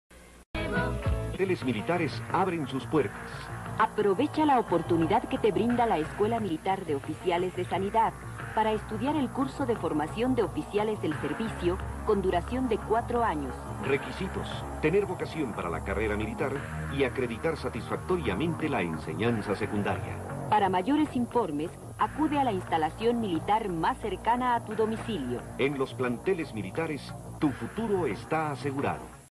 Comerciales mexicanos: SEDENA 1986
Comercial del Ejercito y fuerza aerea entre 1985 y 1986